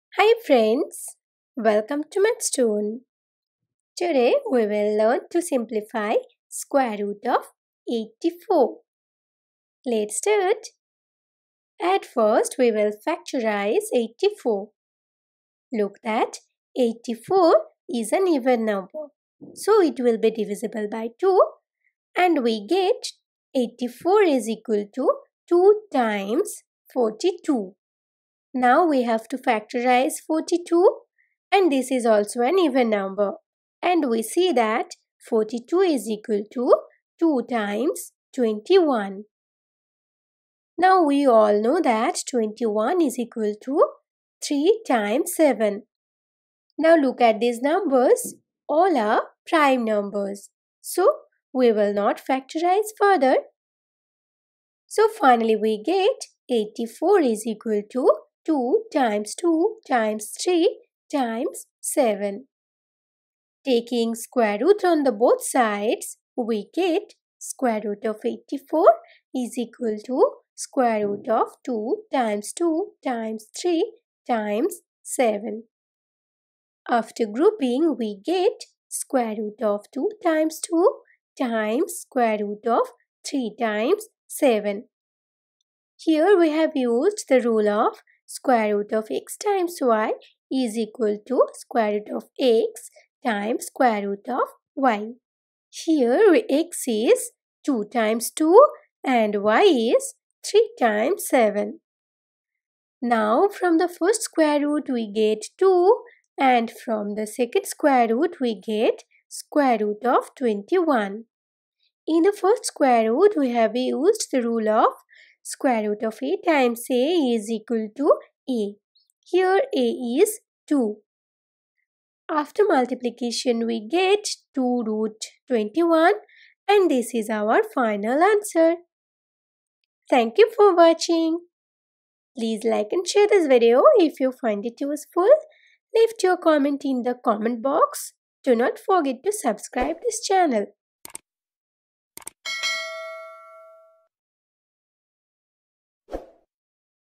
How to simplify square root of 84
In this video, we will find the square root of 84 in a very easy way.

Topic: simplify 84^1/2 | simplify root 84 | simplify square root of 84 | Root 84

We post videos daily on youtube